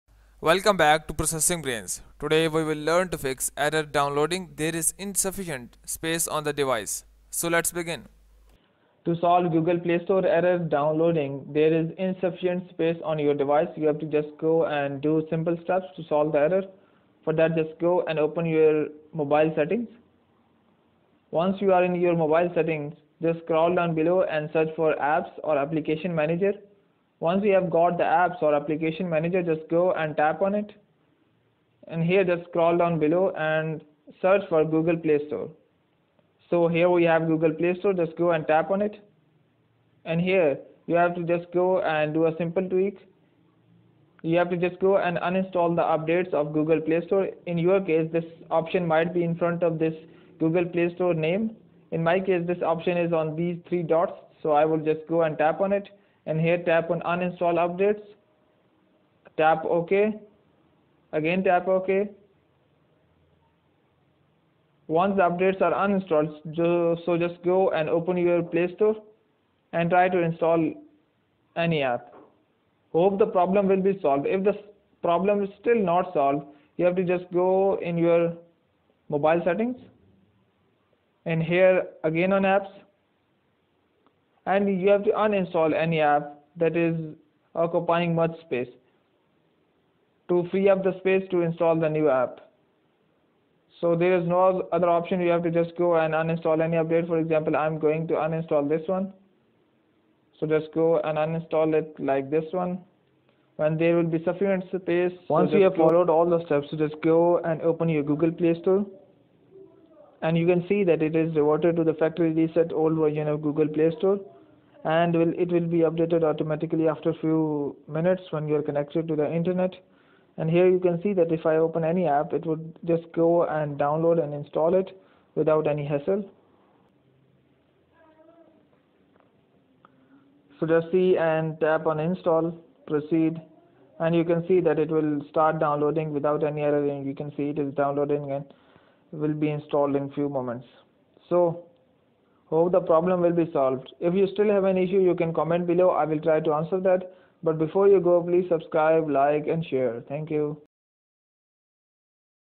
FIX Error Downloading Google Play Services There is Insufficient Space on the Device [UPDATED]
Learn to FIX Error Downloading Google Play Services There is Insufficient Space on the Device [UPDATED]. The error was Error Downloading “App name” There is Insufficient Space on the Device. The point was I was having enough storage on my sd card as well as in internal memory. Then after doing some research I came up with the following methods which I have mentioned below. Most of the time the first method works great but in case if it doesn’t work then we have two back up methods which may work.